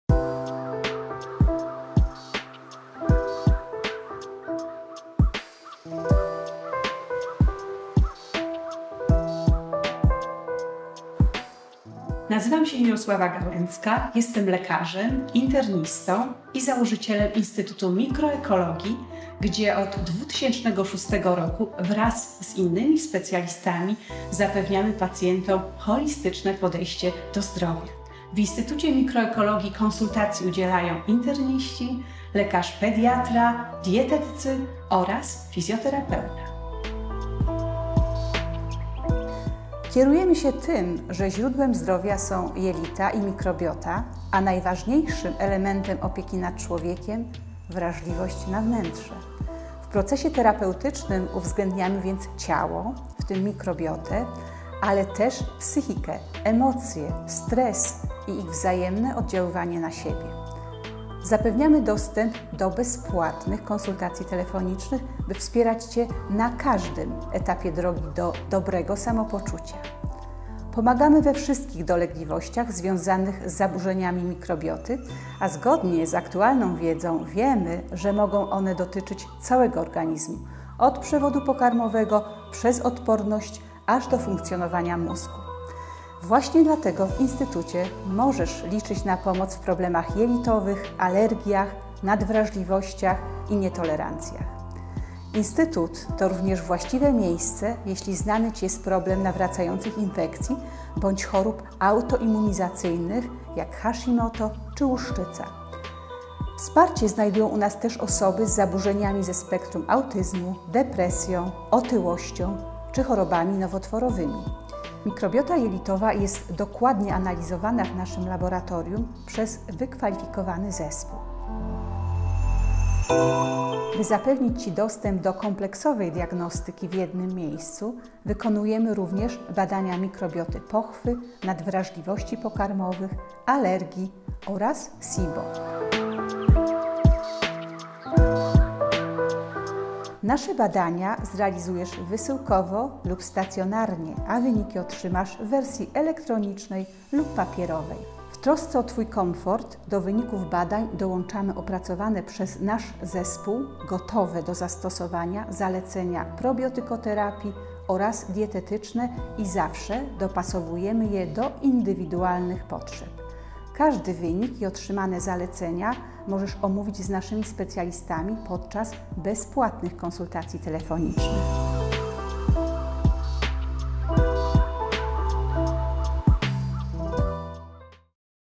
Instytut Mikroekologii - wrażliwi na Twoje wnętrze
Instytut Mikroekologii to miejsce tworzone przez interdyscyplinarny zespół specjalistów: lekarzy, dietetyków, diagnostów laboratoryjnych – wrażliwych na wnętrze. Niezmiennie od niemal dwóch dekad podkreślamy, że zbadanie mikrobioty żyjącej we wnętrzu człowieka pozwala właściwie zaopiekować się zdrowiem. W tym celu wykorzystujemy wysokiej jakości badania diagnostyczne, które oceniają stan mikrobioty jelit i wskazują, jak ją wspierać, by przywrócić równowagę organizmu, zdrowie oraz dobre samopoczucie.